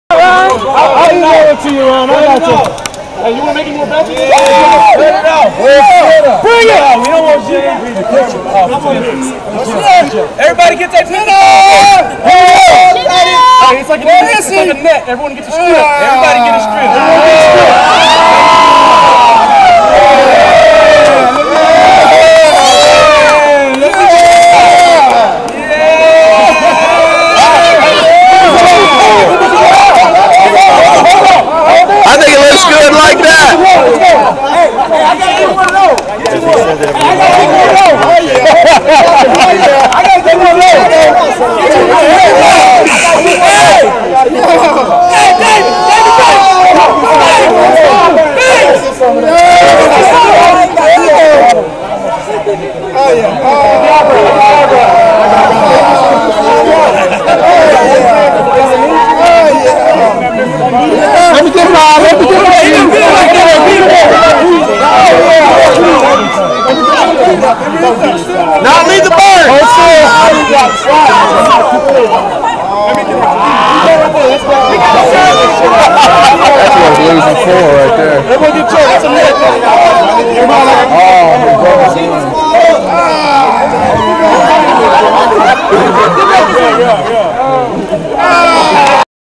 Austin Turfcats GM Oswalt Getting Shaved
Ronald Oswalt getting a nice clean dome after the Turfcats Upset the Swashbucklers on May 9th. 56-53